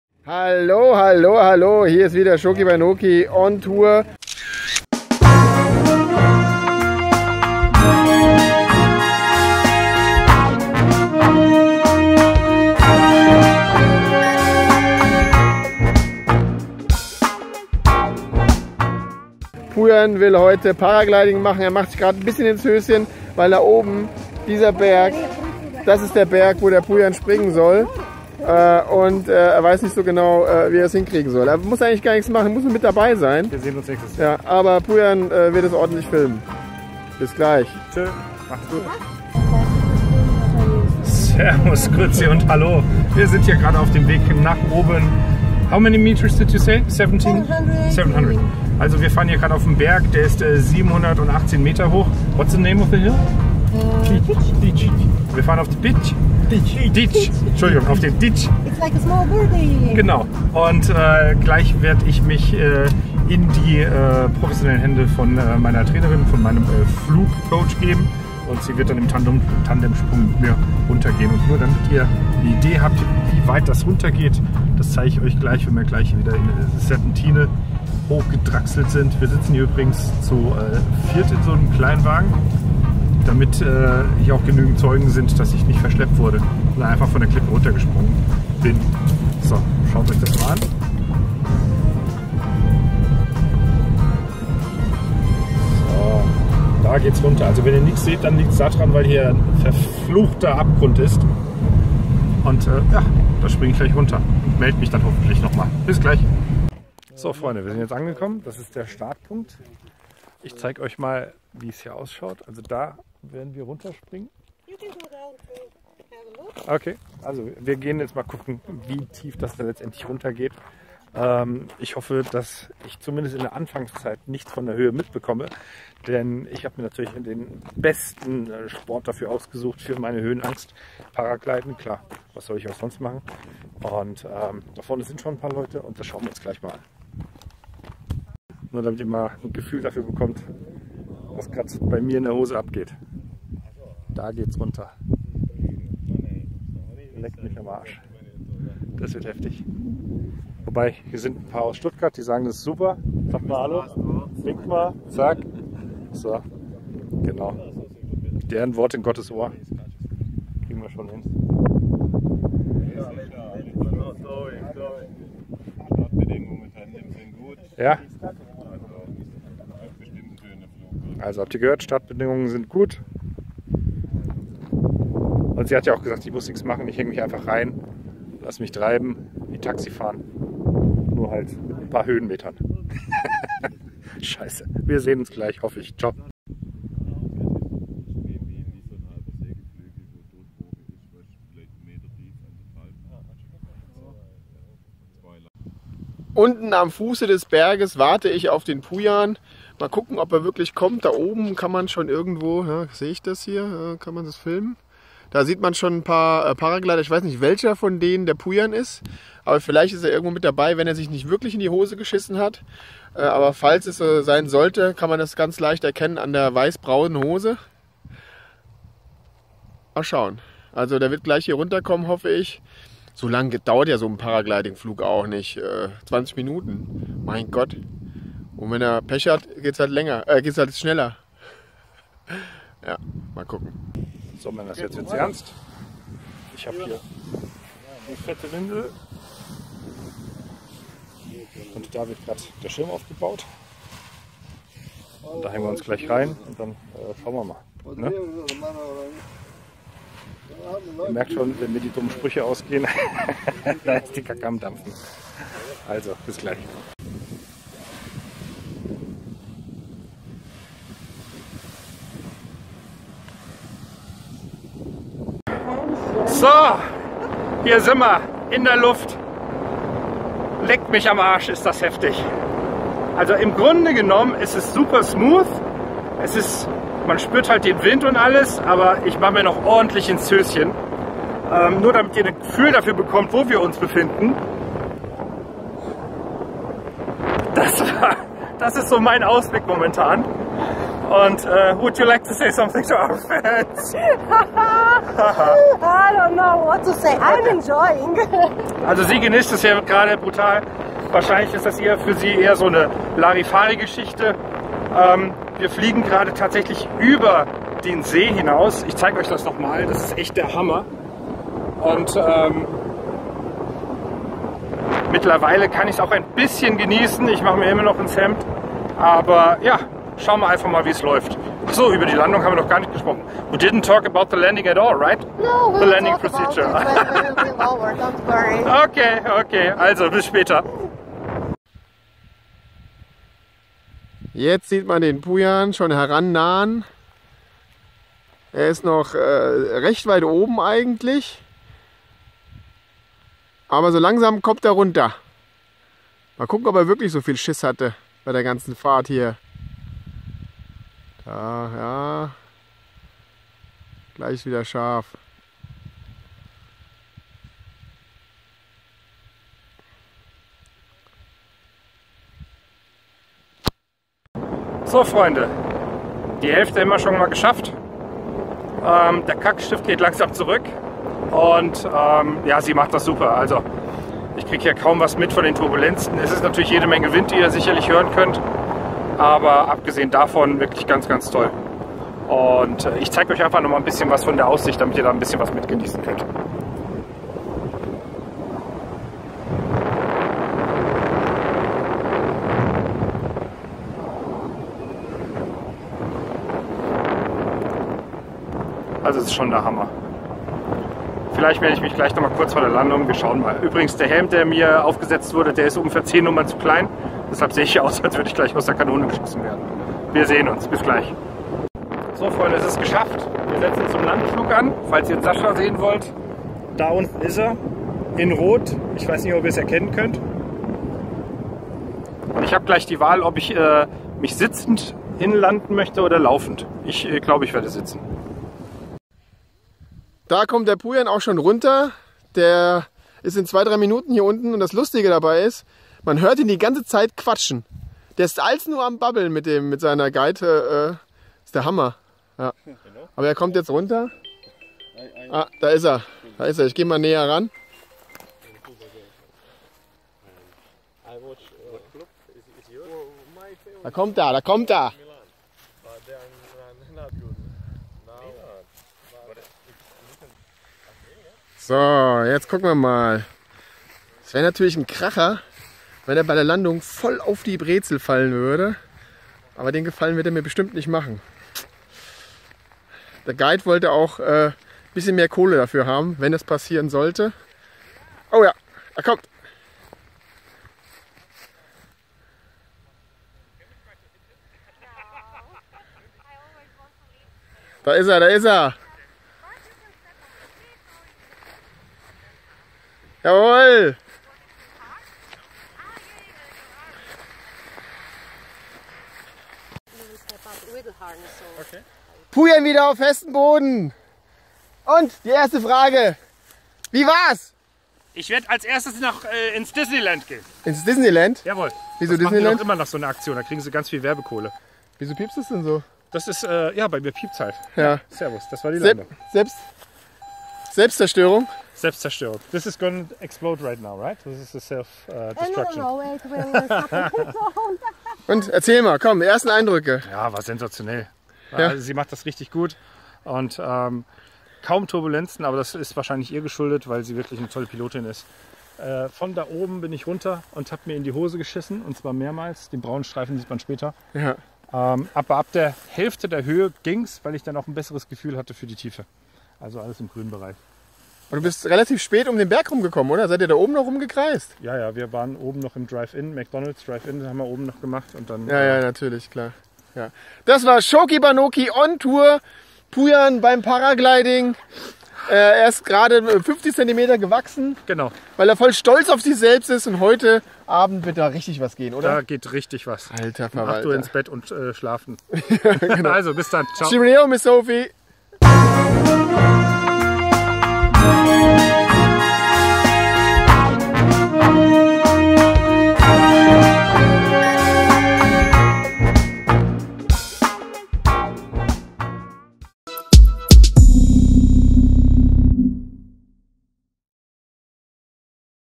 Paragliding in Kroatien | Schoki-Banoki ON TOUR | Pujan in der Luft
Pujan geht heute in die Luft beim Paragliding. Er hat zwar ganz schön die Hosen voll, aber macht es trotzdem. Sascha begleitet ihn von unten … eher moralisch!